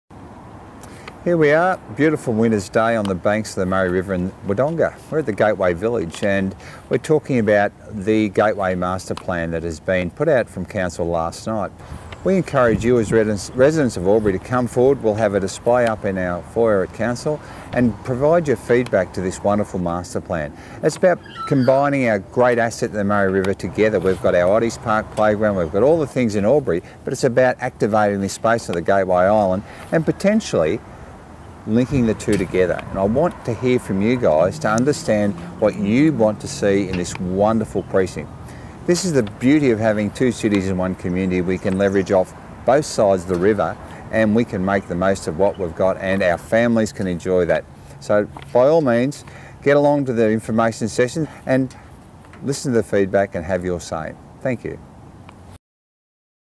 Gateway plan to link border cities
AlburyCity Mayor Kevin Mack urges the community to have a say on Gateway Island master plan